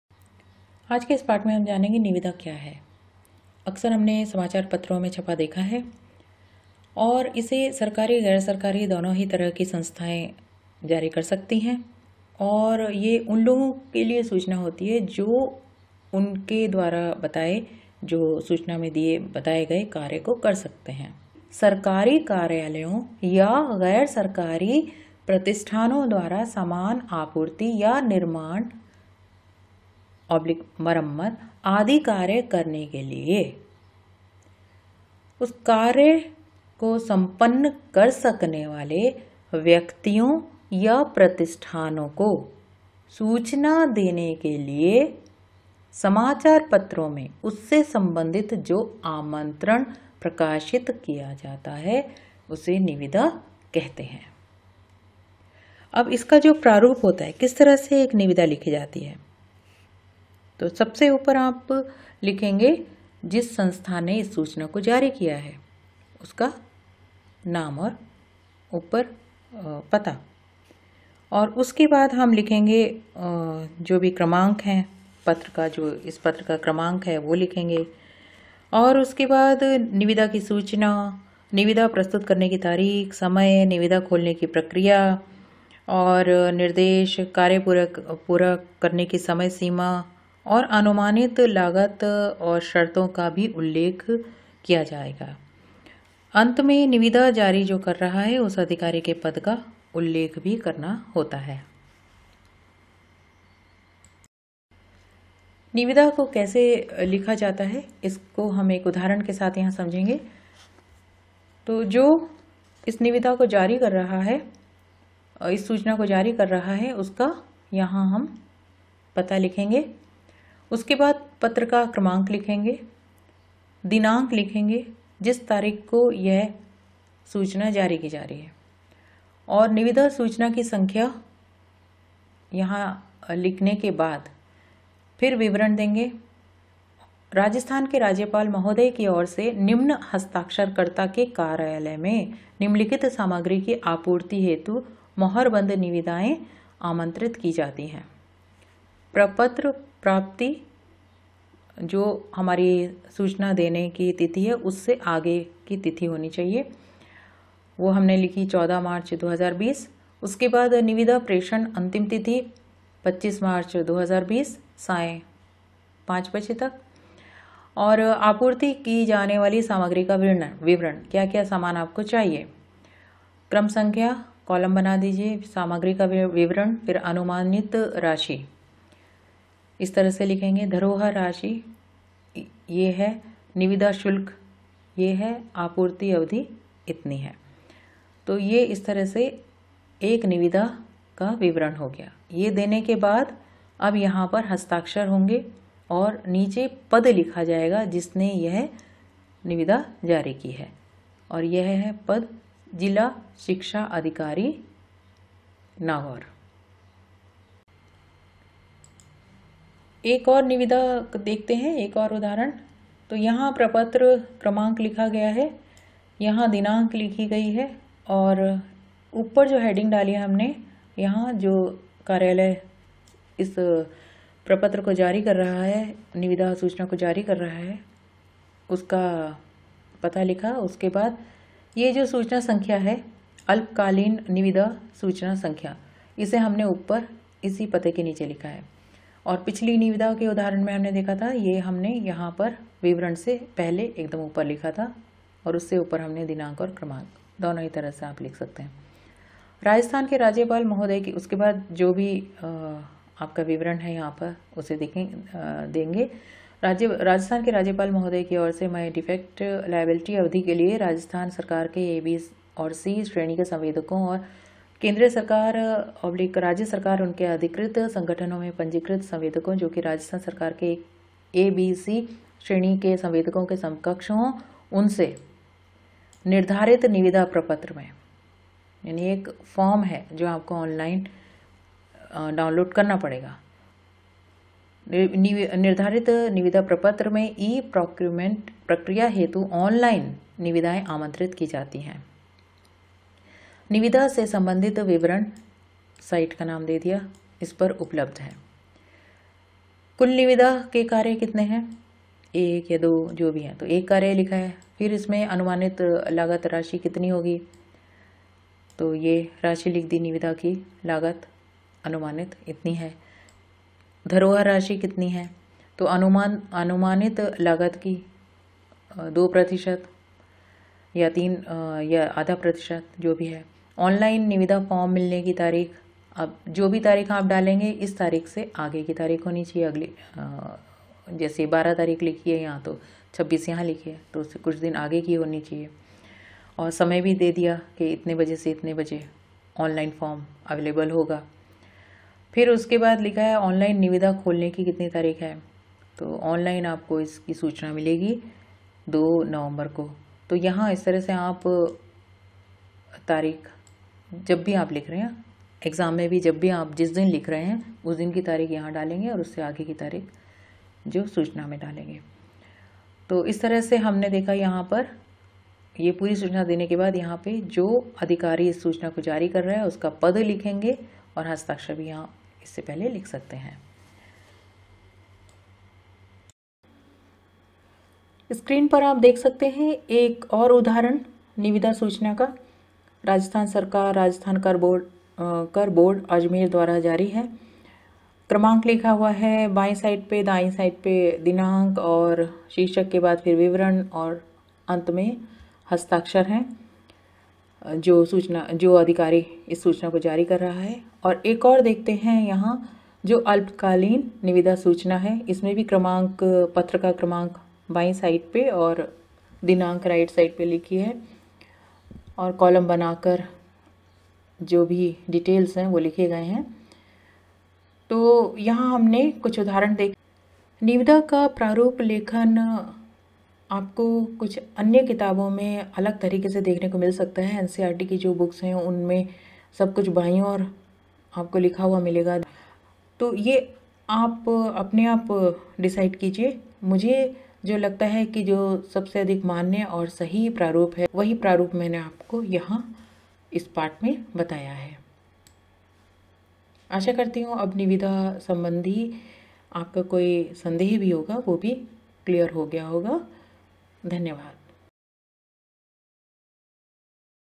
निविदा क्या है /कैसे लिखें Nivida /कार्यालयी लेखन
प्रारूप लेखन  : निविदा /  हिन्दी में निविदा कैसे लिखें/ Learn to write a tender in Hindi
निविदा पत्र लेखन /
निविदा का शाब्दिक अर्थ है, आवश्यक रकम लेकर वांछित वस्तुएँ जुटा देने या काम पूरा करने का लिखित वादा देना। निविदा को अंग्रेजी में Tender (टेण्डर) कहते हैं/
यह एक प्रपत्र होता है जिसमें सामान के आपूर्तिकर्ता द्वारा निर्धारित समय में सामग्री की आपूर्ति हेतु क्रेता को न्यूनतम मूल्य बताये जाते हैं अर्थात् न्यूनतम दर पर समान की आपूर्ति के आश्वासन पत्र को निविदा पत्र कहा जाता हैI यह पत्र आपूर्तिकर्ता द्वारा सम्भावित क्रेताओं के समक्ष स्वत: प्रस्तुत किए जाते हैंI
Video lesson prepared and presented by Alpana Verma